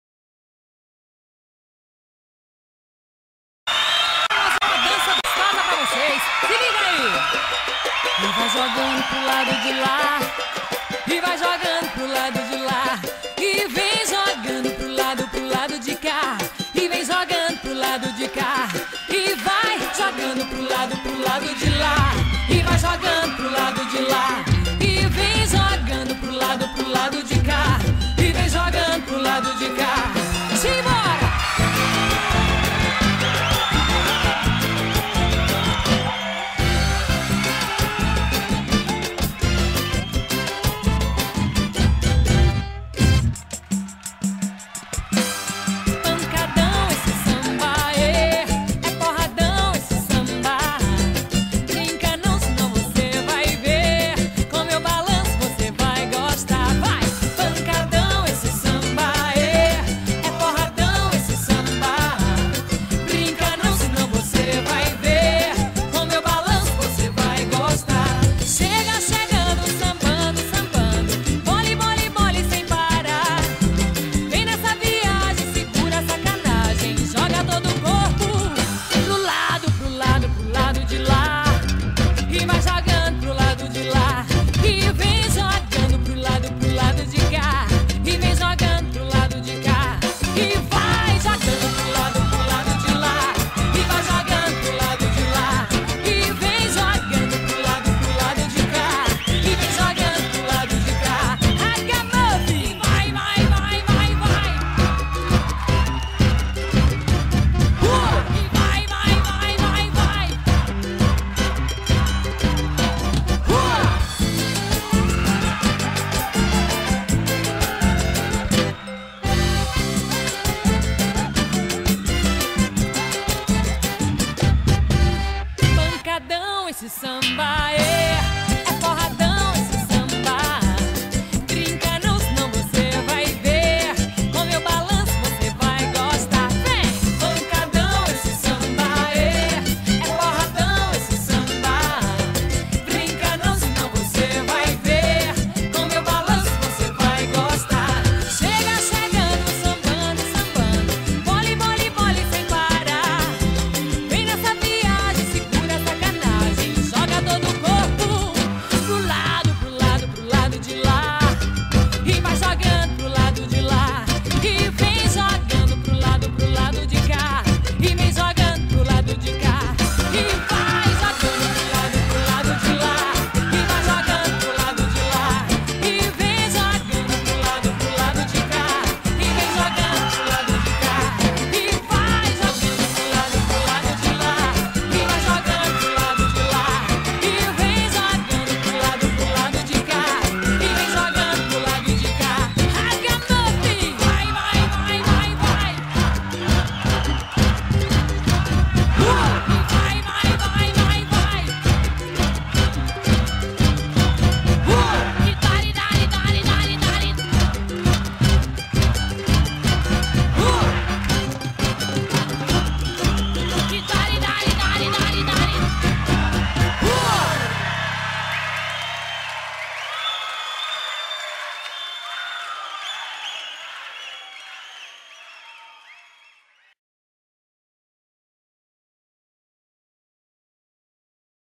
Axé Loca_Pancadao
Axé Loca performing Pancadao at Gossip Bar and Kalamis Naturel Festival...